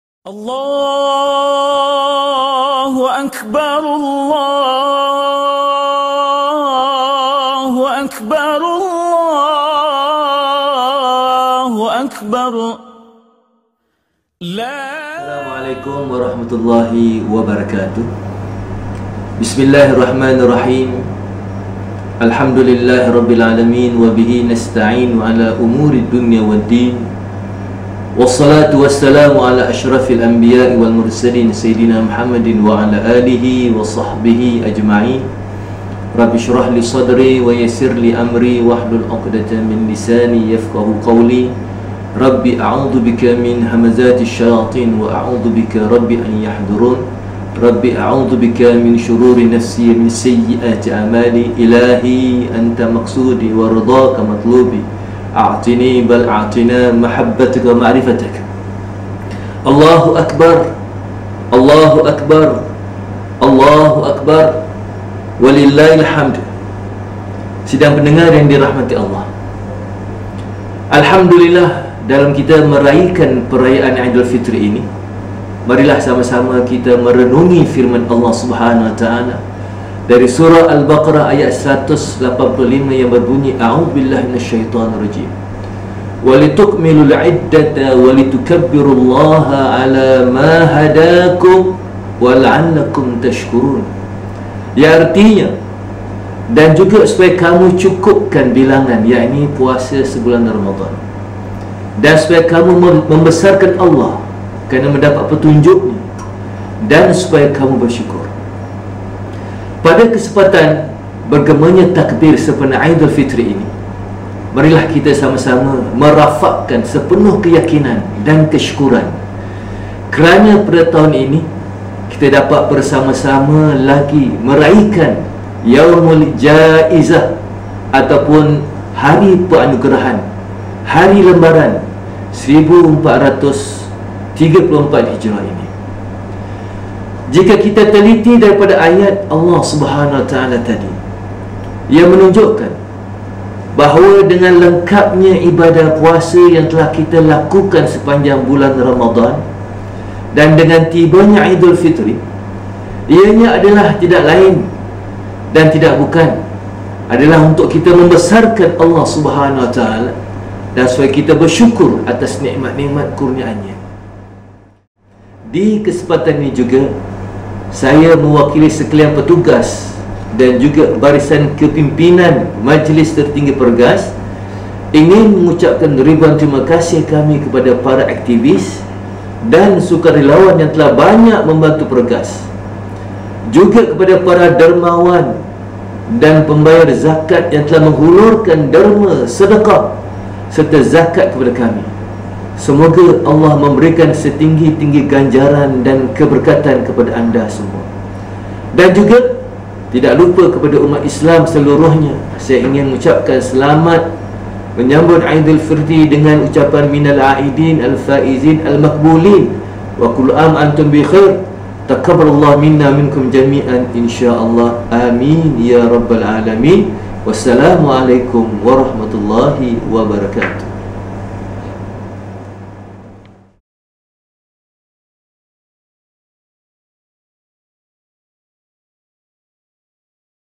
Amanat Aidilfitri 1434H
Al-Fadhil Al-Ustaz Mohamad Hasbi Hassan memberi amanat Aidilfitri 1434H.

Minal `āidīn Wal Fāizīn Wal Maqbūlīn Kullu `āmin wa Antum Bikheir.